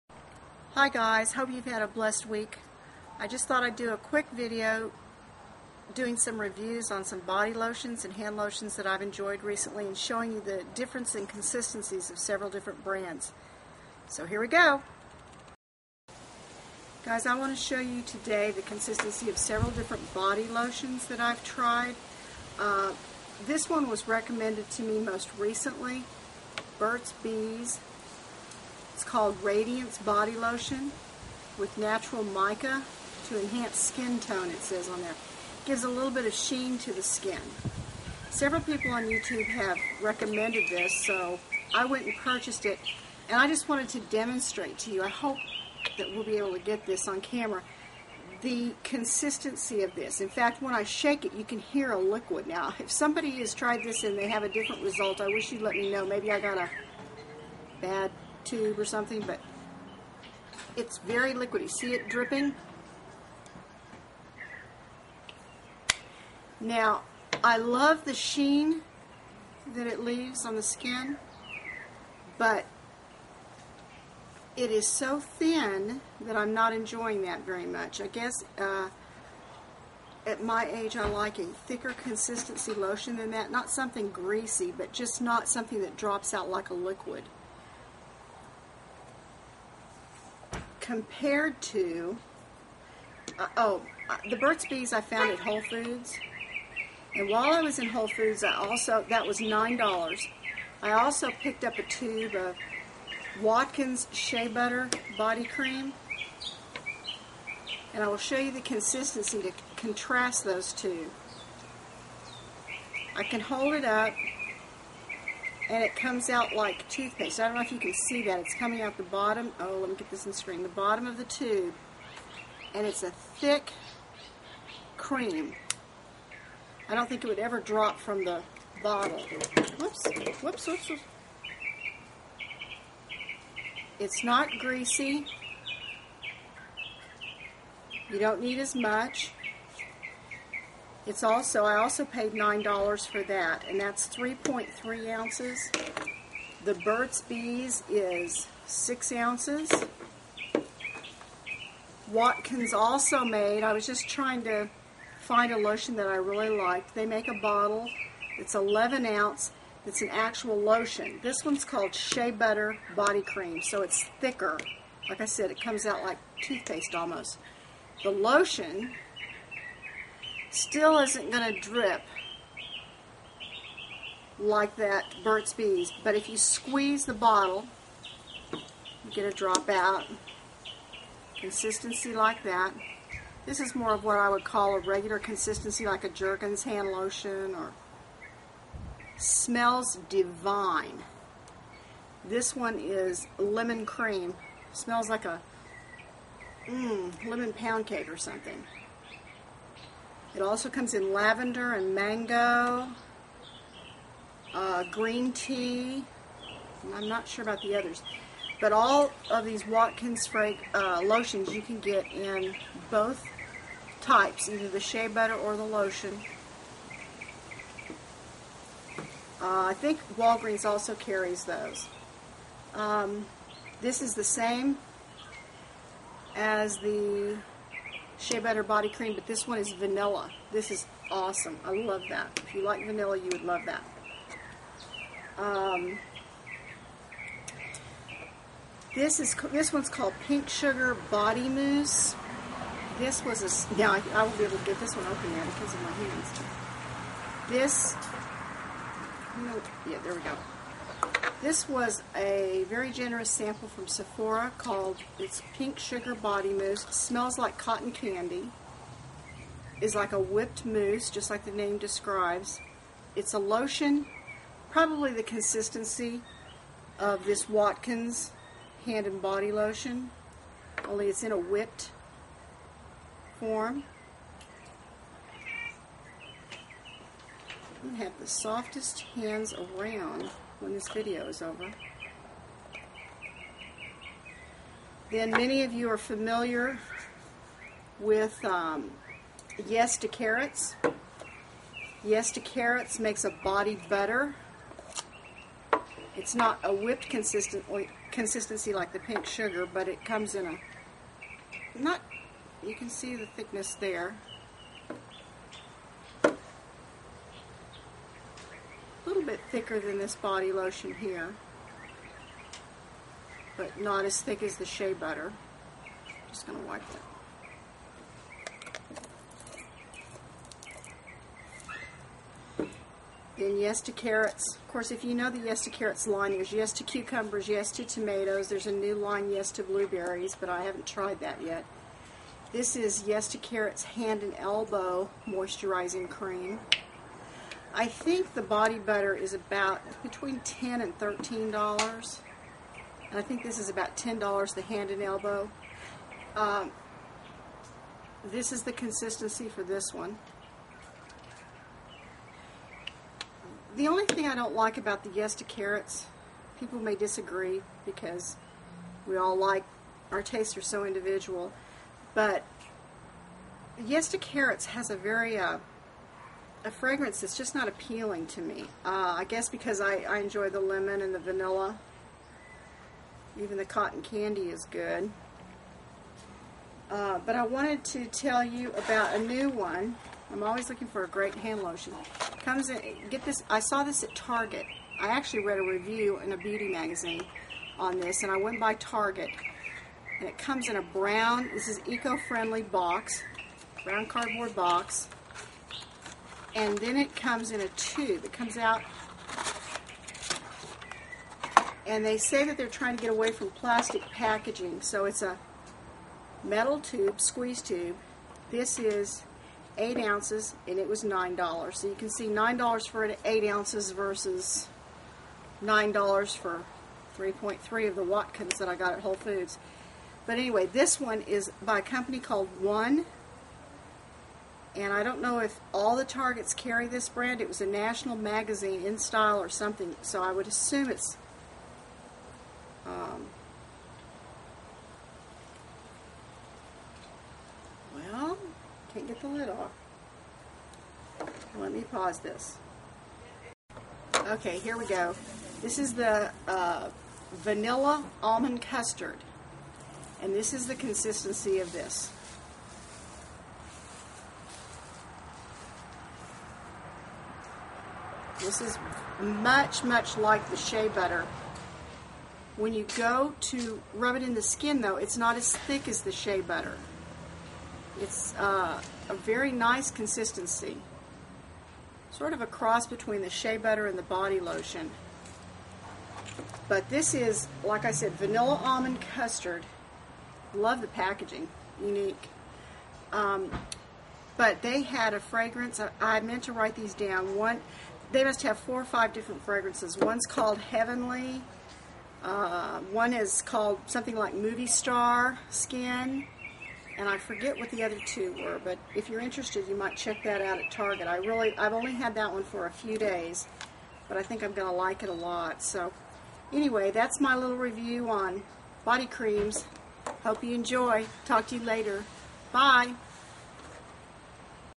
Review of Burt's Bees, Yes to Carrots, Watkins and other Body Lotions
Demonstration of different consistencies of body lotions.